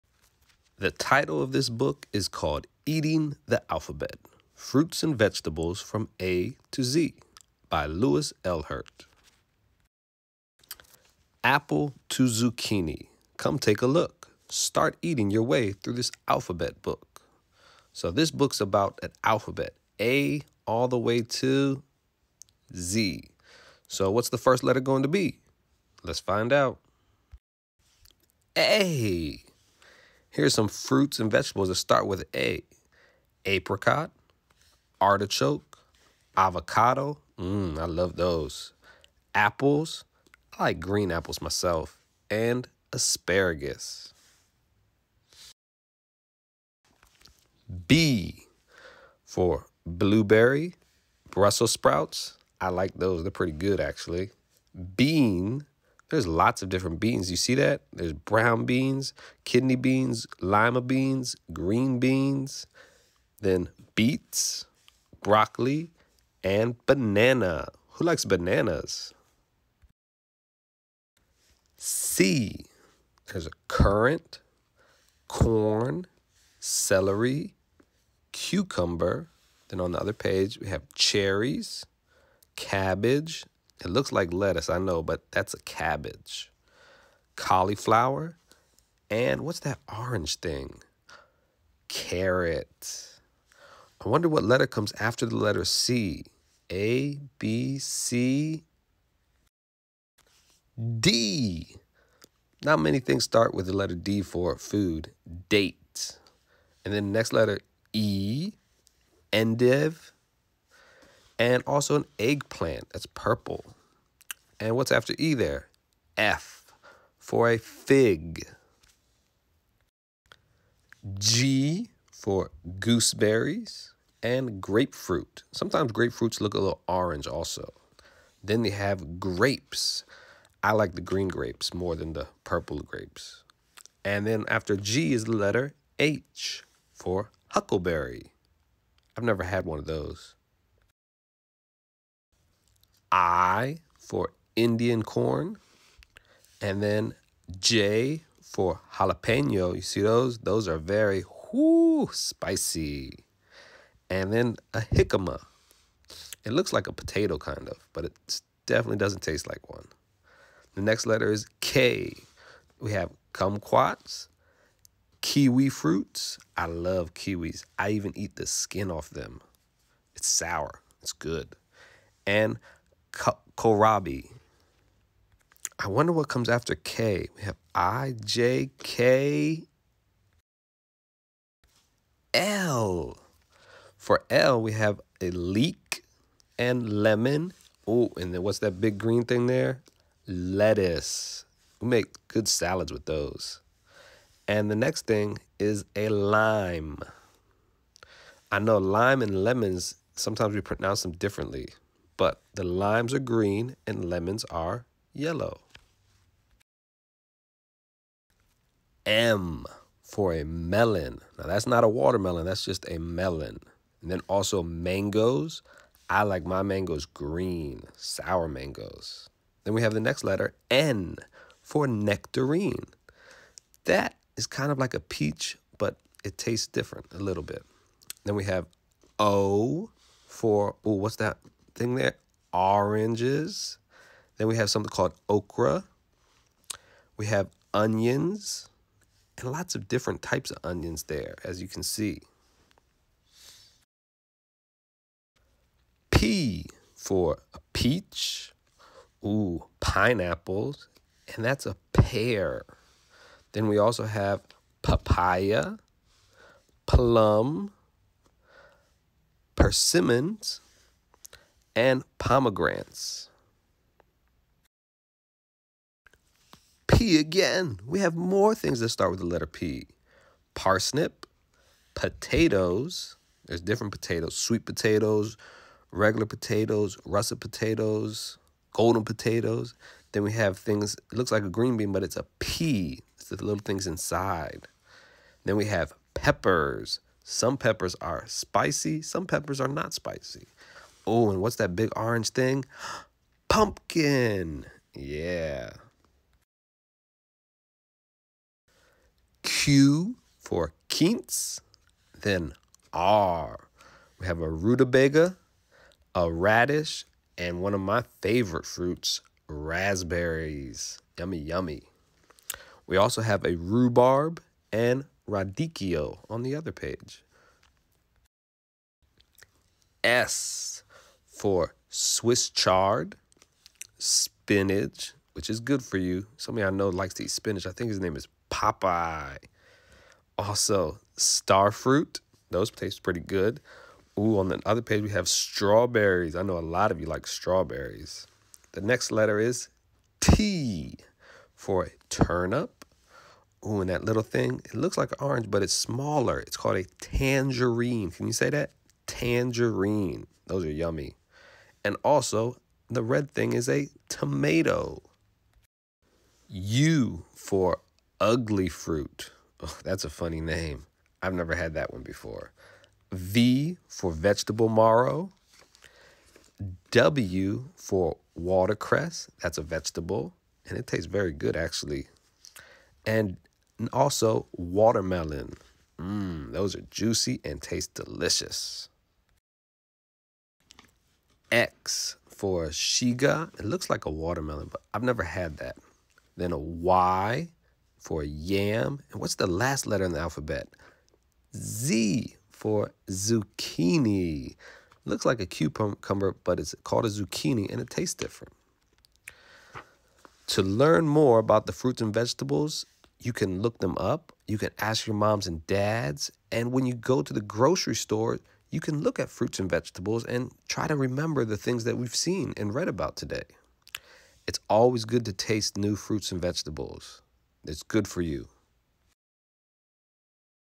Mr. Lyst reading Eating the Alphabet
This is an ABC book about different friits and vegeables that are delicious and good for you.

When finished reading this story think of your favorite fruit or vegetable and write a sentence “I like ______” if you like more than one make another sentence as well. Dont forget to draw a picute of you eating it